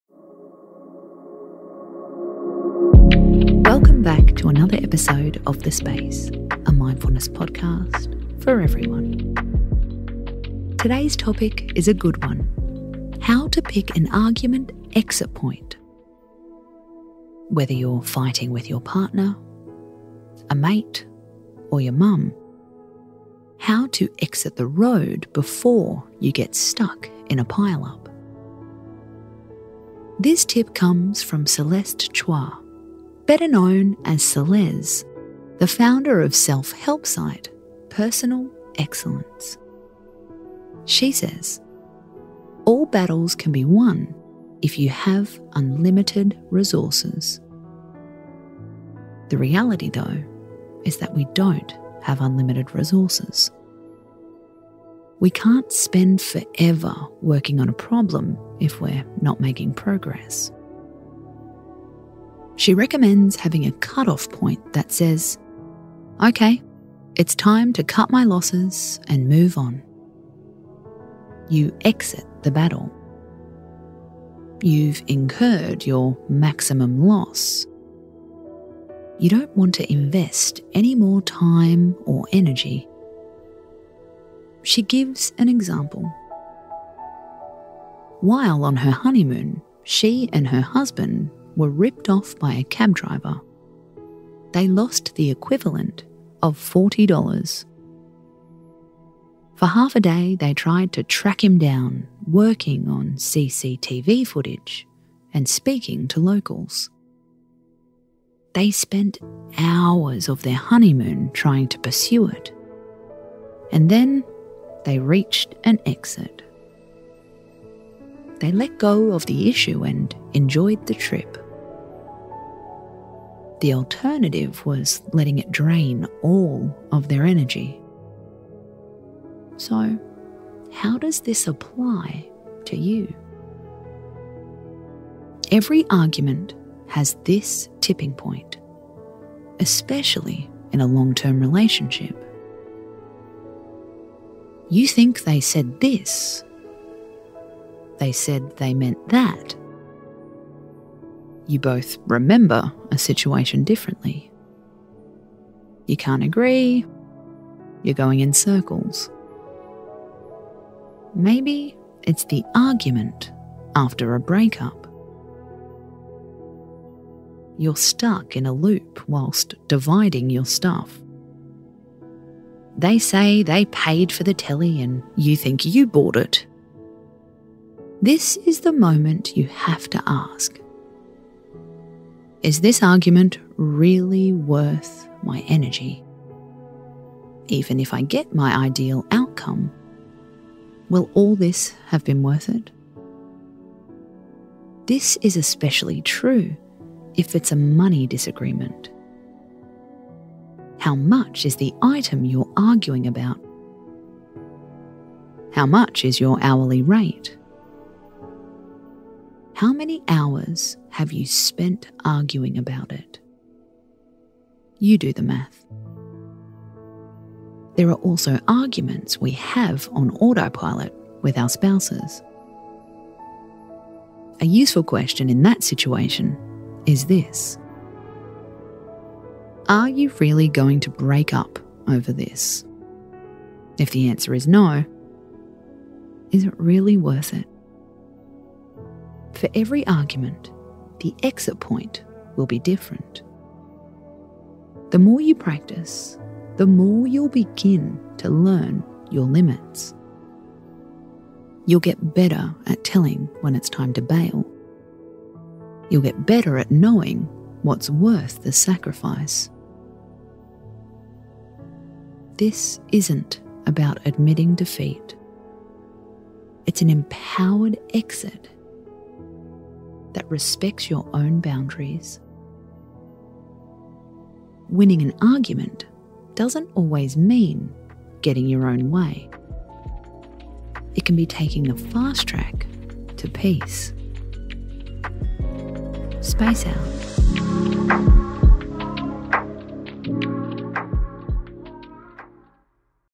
Is this argument REALLY worth my energy?
Using tips from Celes Chua we’re going to teach you how to exit an argument before it becomes a full-on shit show.

LINKS

founder of Personal Excellence.

CREDITS
Host: Casey Donovan @caseydonovan88